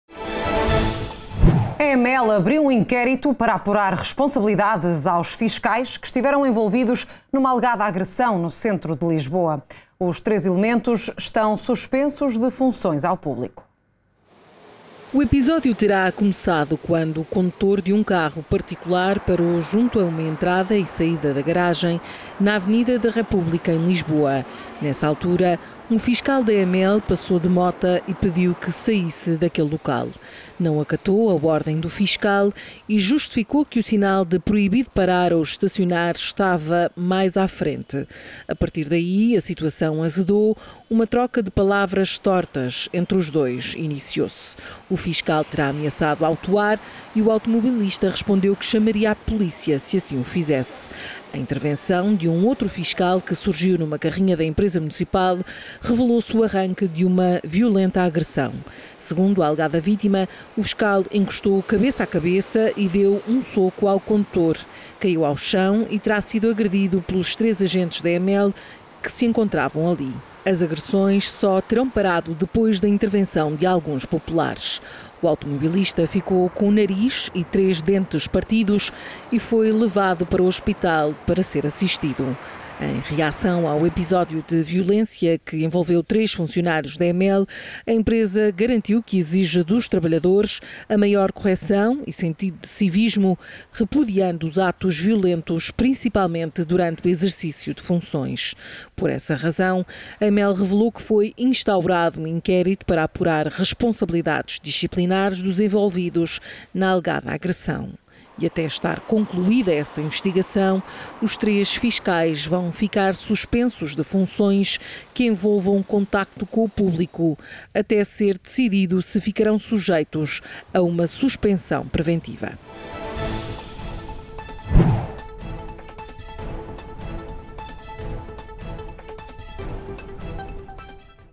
Agressão em Lisboa: EMEL abre inquérito
A EMEL abriu um inquérito para apurar responsabilidades aos fiscais que estiveram envolvidos numa alegada agressão, no centro de Lisboa. Os três elementos estão suspensos de funções ao público.